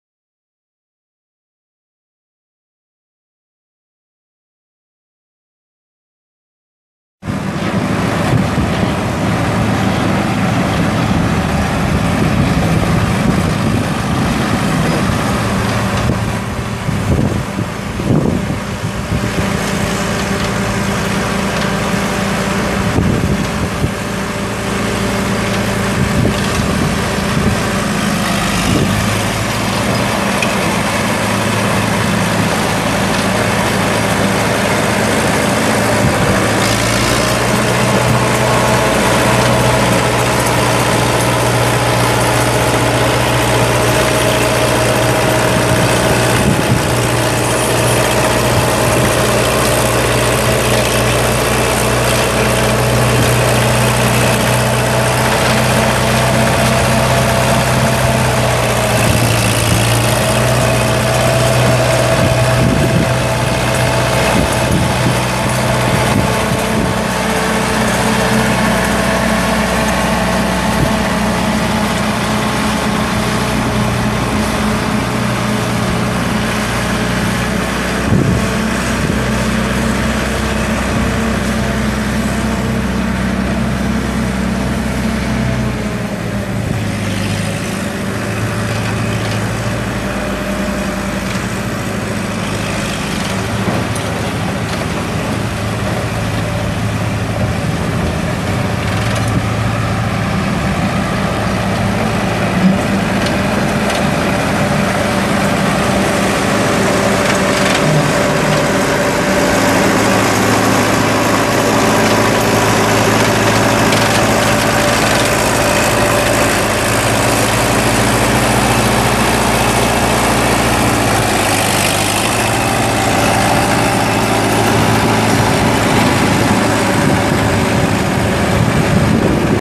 1951 Caterpillar 112 rigid frame motor grader for sale | no-reserve auction September 29, 2016
1951 Caterpillar 112 rigid frame motor grader
Caterpillar four cylinder diesel engine
Pony motor
Direct-drive transmission
6F-2R gears
EROPS
12'W moldboard
13.00-24 tires
Pony motor only fires on one cylinder
Pony motor needs work
Serial 3U1699

Item location address: This item is located at 13545 Onaga Road, Onaga, KS

It's located in Onaga, KS, and we're selling it to the highest bidder regardless of price.